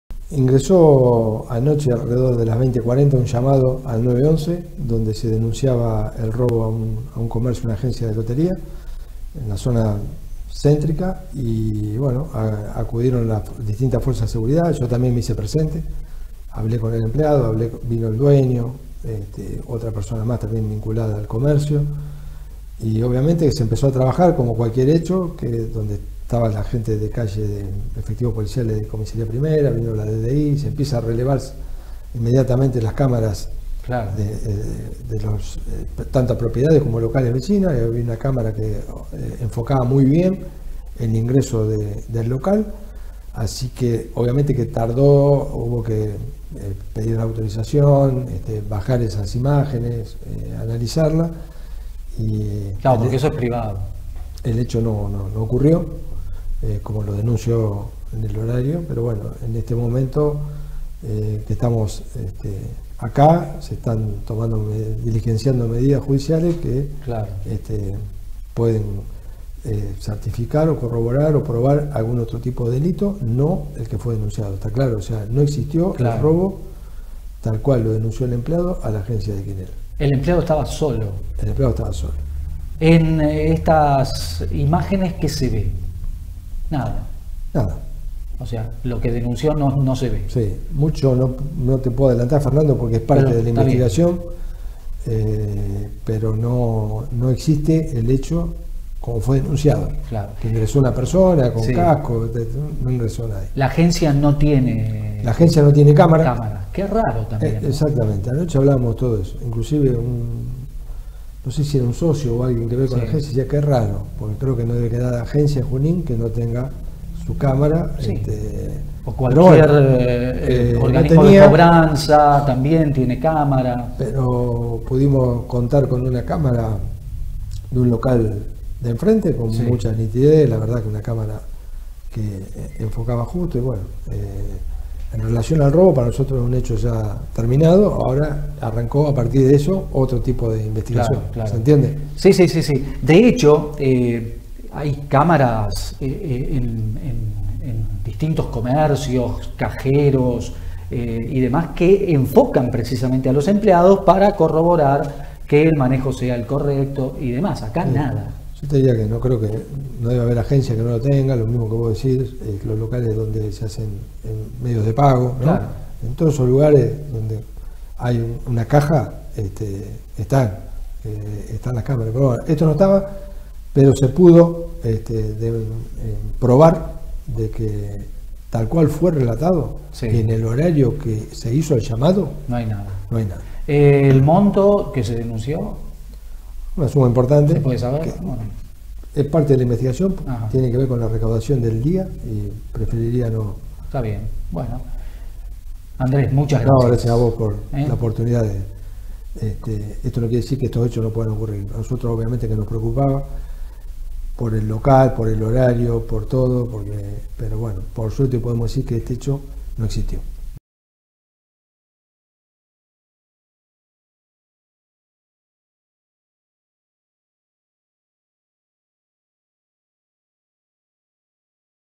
INVESTIGAN *FALSA DENUNCIA*
Robo a agencia.
"El hecho no ocurrió como lo denunció."
"No existió el robo tal cual lo denunció el empleado."
Dr. Andrés Rosa - Secretario de Seguridad